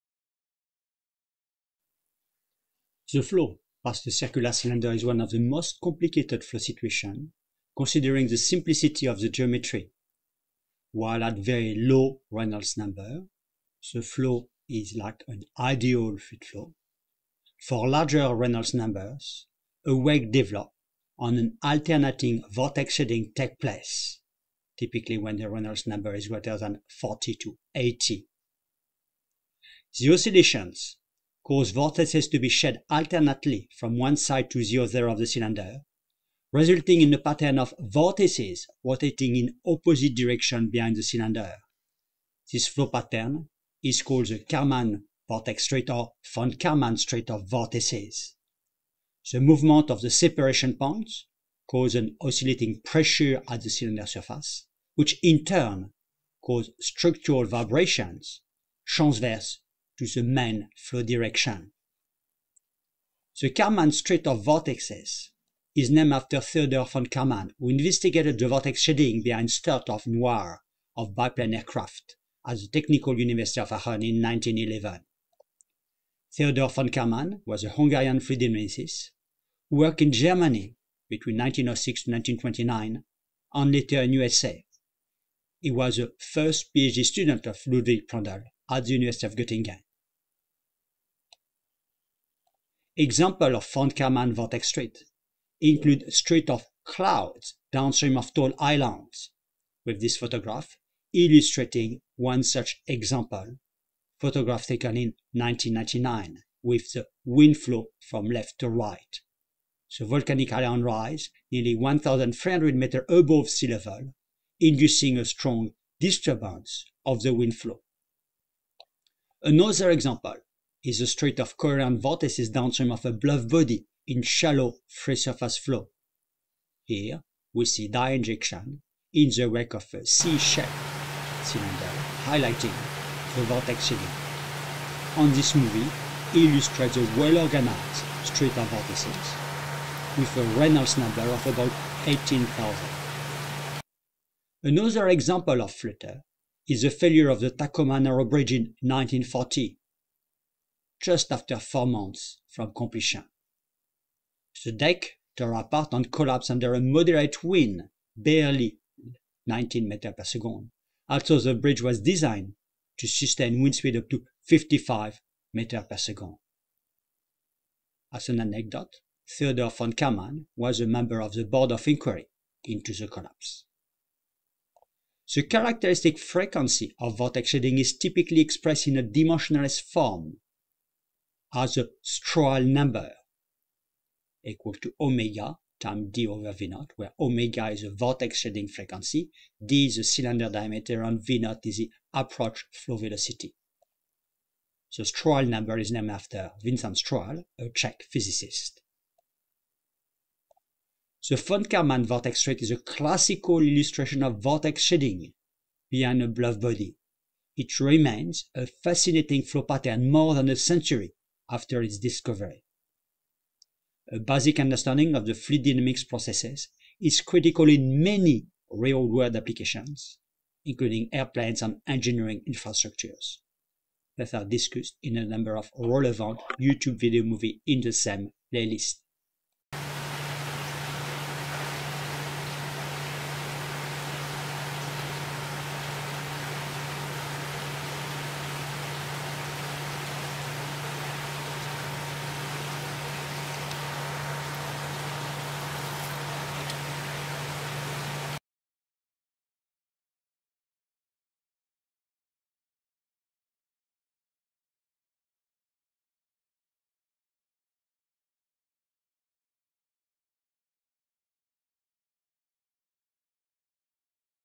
Von Karman Street of Vortices (1) Presentation
The flow past a circular cylinder is one of the most complicated flow situations considering the simplicity of the geometry. While the flow field is nearly ideal for low Reynolds numbers (ReD less than 1), a wake develops at larger Reynolds numbers, and an alternating vortex shedding takes place for ReD more than 40 to 80. The oscillations cause vortices to be shed alternately from one side to the other of the cylinder, resulting in a pattern of vortices rotating in opposite directions behind the cylinder, and this flow pattern is called a von Karman street of vortices, or von Karman vortex street. The movement of the separation points causes an oscillating pressure at the cylinder surface which in turn may cause structural vibrations transverse to the free-stream.
The Karman vortex street, or von Karman street of vortices, is named after Theodore von Karman, who investigated the vortex shedding behind sturt and wire of biplane aircrafts at the Technical University of Aachen, Germany in 1911.
Examples of von Karman vortex street include street of clouds downstream of tall islands. This photograph illustrates on the von Karman street of clouds downstream of Alexander Selkirk Island in the southern Pacific Ocea on 15 September 1999, with wind flow from right to left. The volcanic island rises 1,268 m above sea level, inducing a significant disturbance to wind flow.
Another example is the street of coherent vortices downstream of a bluff body in shallow free-surface flow. In this photograph, dye injection in the wake of the C-shaped cylinder highlights the vortex shedding.
Another example of flutter is the failure of the Tacoma Narrows bridge in 1940. After just four months behind completion, the deck tore apart and collapsed under a moderate wind (i.e. 19 m/s) although the bridge was designed to withstand gales of 55 m/s. Motion pictures taken of the disaster show the deck rolling up and down and twisting wildly, because of aerodynamic flutter. For completeness, Theodore von Kármán was a member of the board of inquiry into the collapse.
The von Karman vortex street is a classical illustration of vortex shedding behind a bluff body. It remains a fascinating flow pattern, more than a century after its discovery.
A basic understanding of the fluid dynamics processes is critical in many real-word applications including airplanes and engineering infrastructures. These discussed in a number of relevant Youtube video movies in the same Playlist at:

Acknowledgements
Prof. Colin J. APELT
USGS EROS Data Center Satellite Systems Branch
NASA Earth Observatory

References
VALLENTINE, H.R. (1969). "Applied Hydrodynamics." Butterworths, London, UK, SI edition.
SCHLICHTING, H. (1979). "Boundary Layer Theory." McGraw-Hill, New York, USA, 7th edition.
VAN DYKE, M. (1982). "An Album of Fluid Motion." Parabolic Press, Stanford CA, USA, 176 pages.
CHANSON, H. (2014). "Applied Hydrodynamics: An Introduction." CRC Press, Taylor & Francis Group, Leiden, The Netherlands, 448 pages & 21 video movies (ISBN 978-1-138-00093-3).